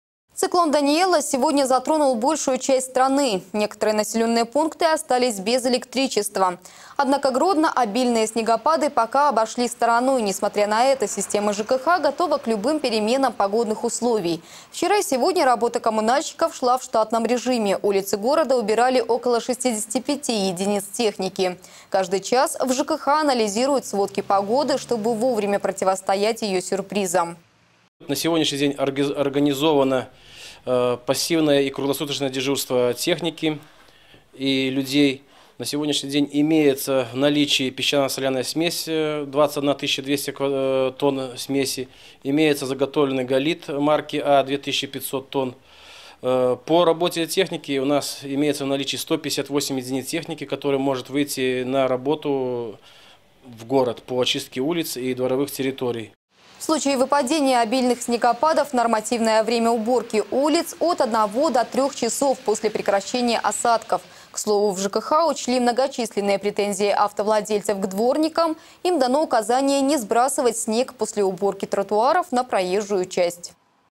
ЖКХ снегопад
Циклон «Даниелла» не затронул Гродно. Коммунальщики следят за погодой и готовы в любую минуты выехать на уборку снега, подробности на сайте